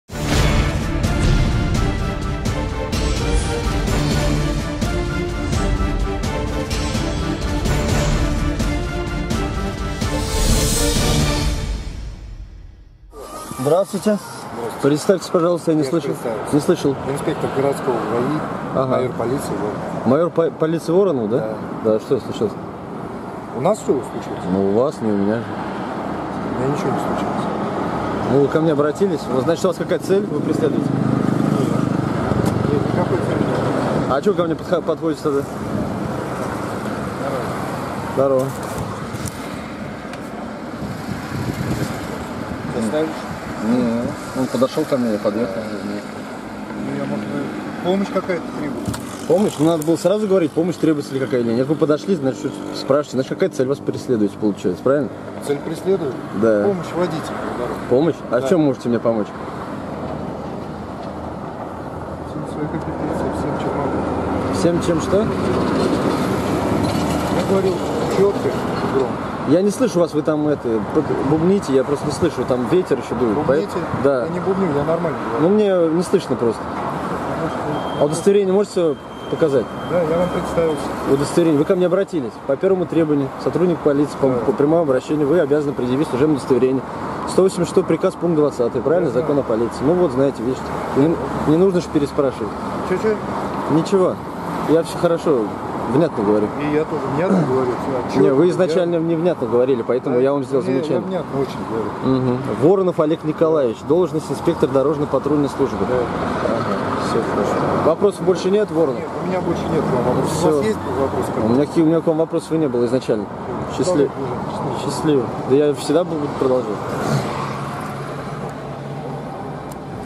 ГАИ Энгельса. Майор Воронов "помогает" водителям
Открытое обращение в контролирующие и надзорные органы МВД ГИБДД по Саратовской области.